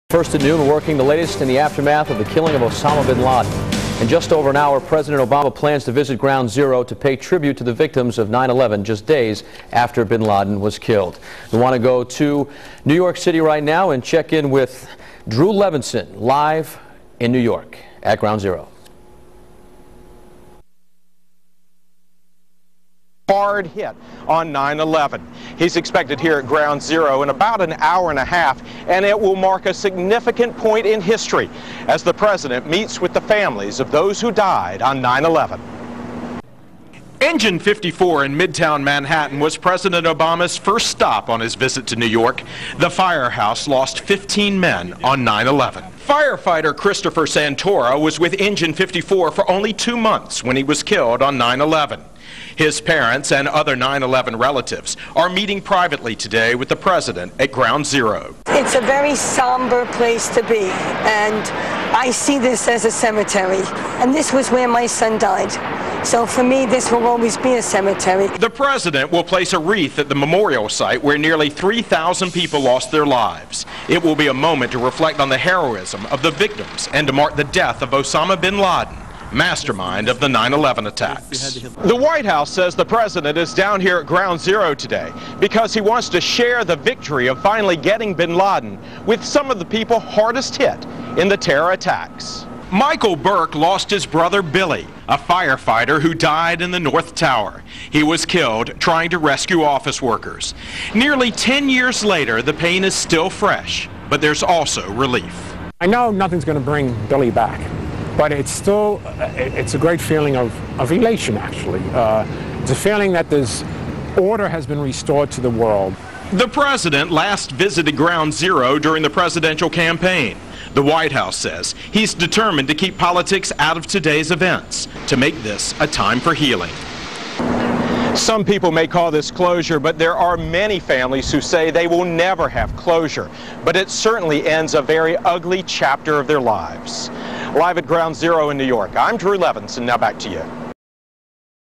President Obama at Ground Zero
President Obama meets with 9/11 victims' families at Ground Zero Thursday.